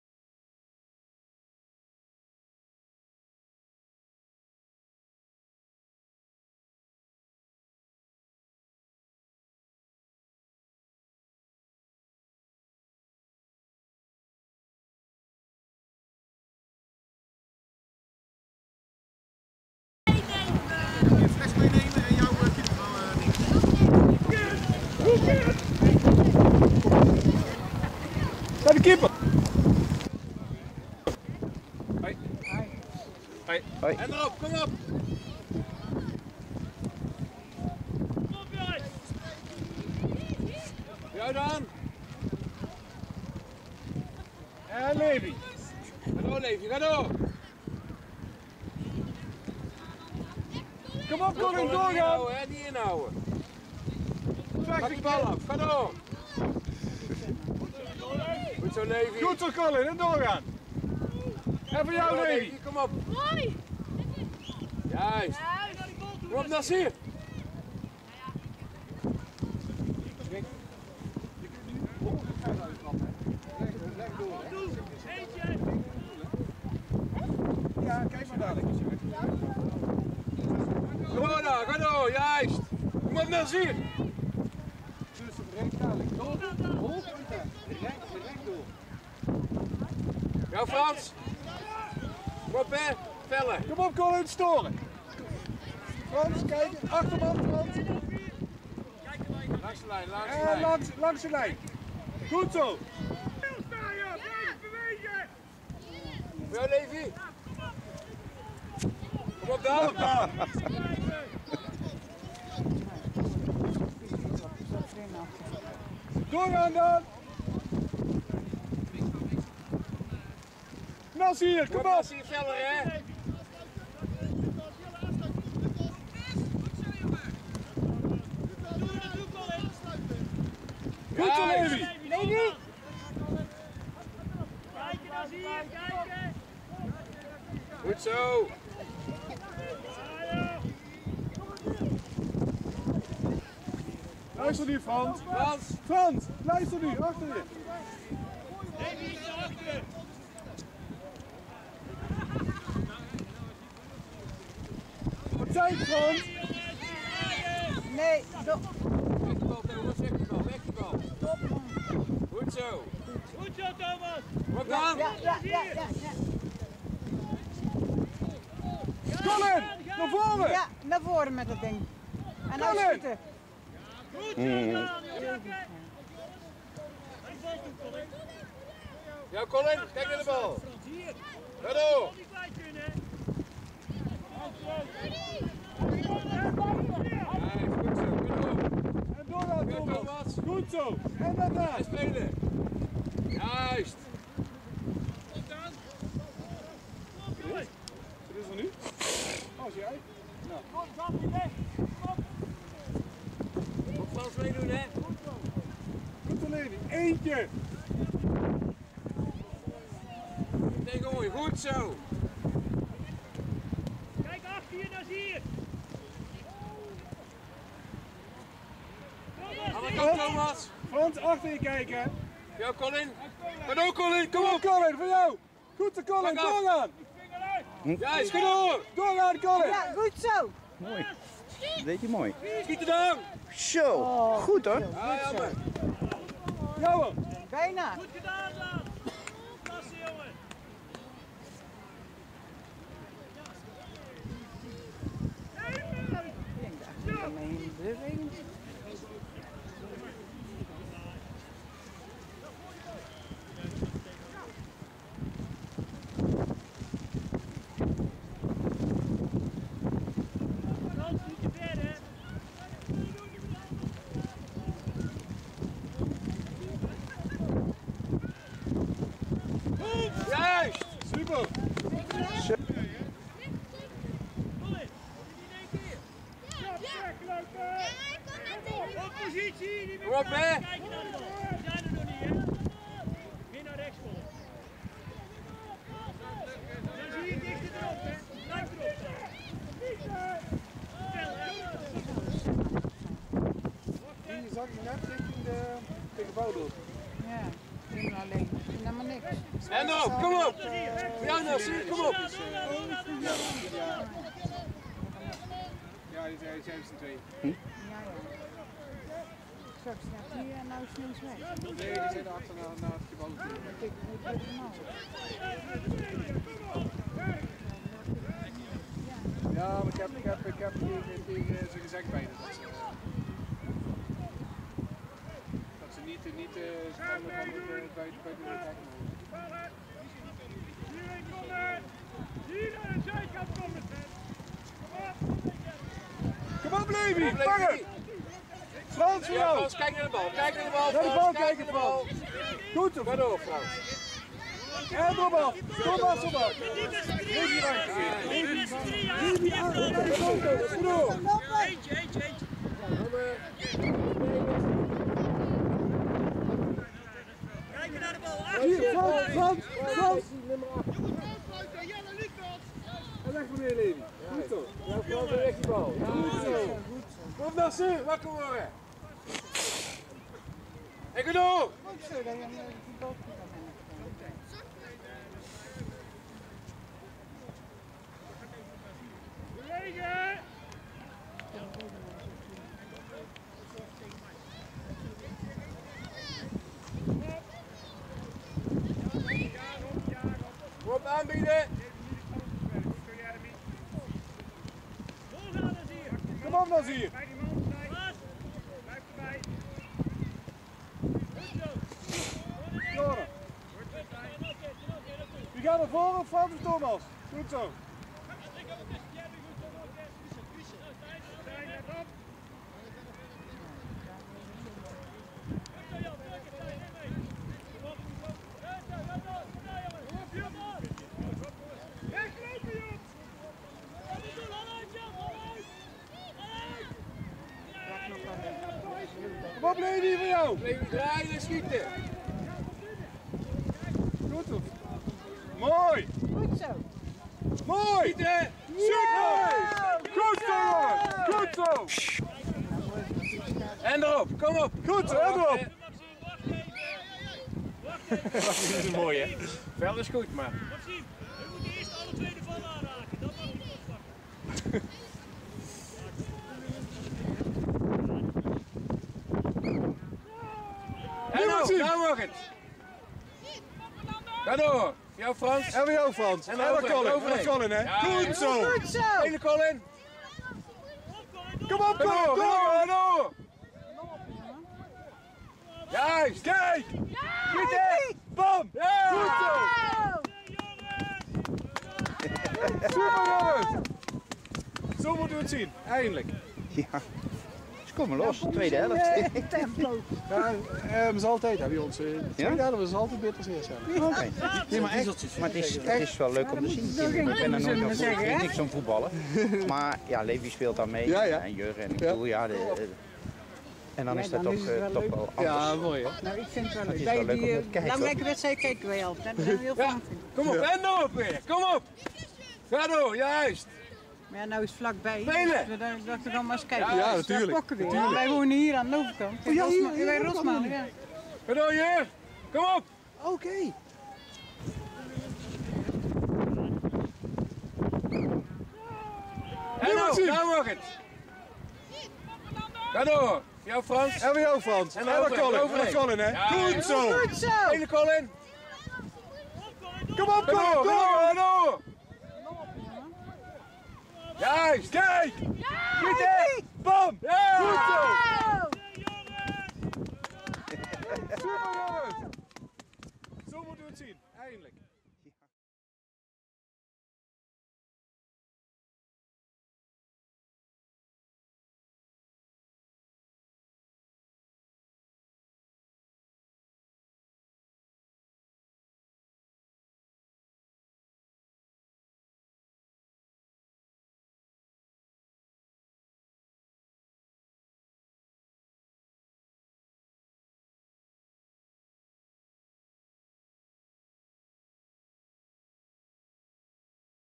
2014-2-8 OJC ROSMALEN E12 FC Engelen E6 tweede helft
Voetbaltoernooi voor de jeugd op het terrein van OJC Rosmalen, waar ik een beeldverslag maakte van de wedstrijd OJC ROSMALEN E12   FC Engelen E6.
De wedstrijd werd gewonnen door FC Engelen met 3 - 5